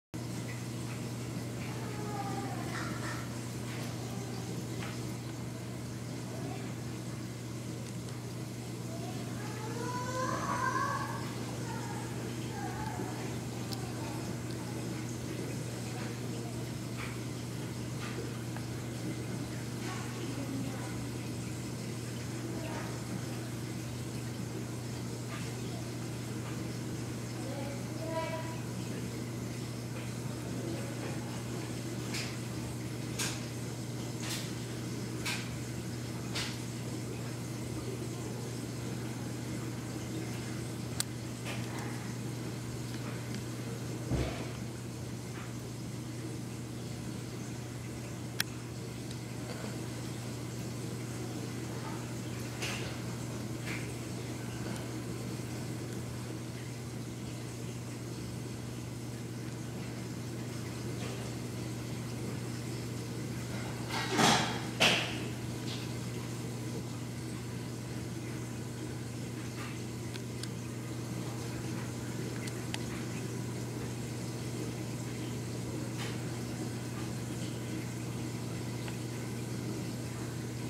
Tail Spot Blenny checking out his new neighbor
After I recorded this video, the 2 tail spot blennies are getting along fine so far.  I plan to get a few barnacle blennies to join the family.